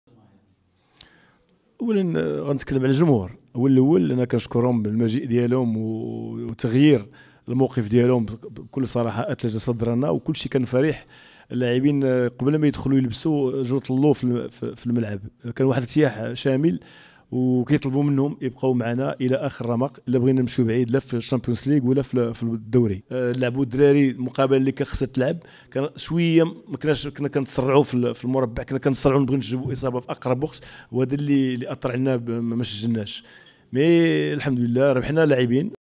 البطولة_الوطنية| عبد الرحيم طالب الحضور الجماهيري الكبير زاد من معنويات اللاعبين .
البطولة_الوطنية| عبد الرحيم طالب الحضور الجماهيري الكبير زاد من معنويات اللاعبين ونتمنى بقاء الحال على هذا الشكل .